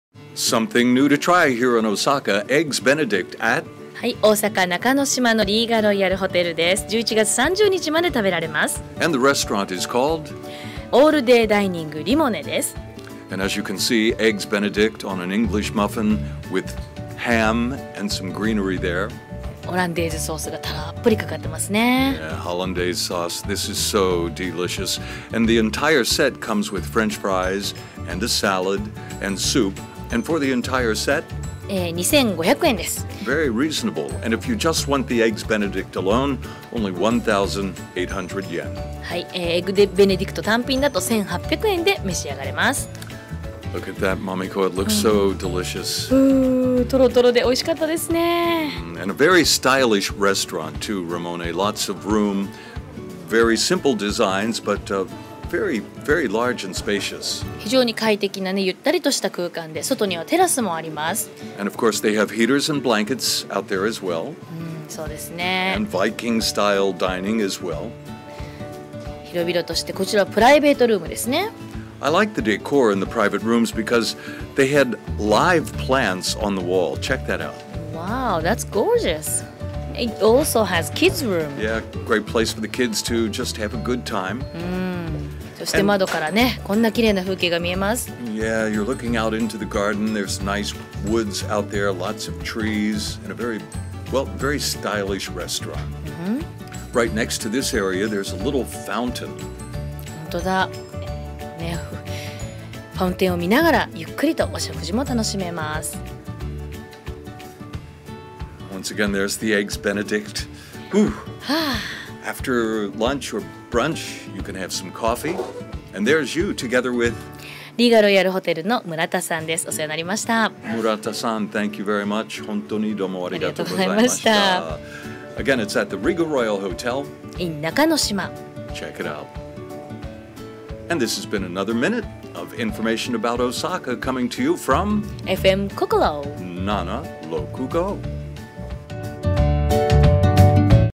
Eggs Benedict at Rhiga Royal Hotel in Osaka
There's an Eggs Benedict boom going on in Osaka right now, and perhaps one of the best places to enjoy a plateful is in the beautiful Rhiga Royal hotel on Nakanoshima.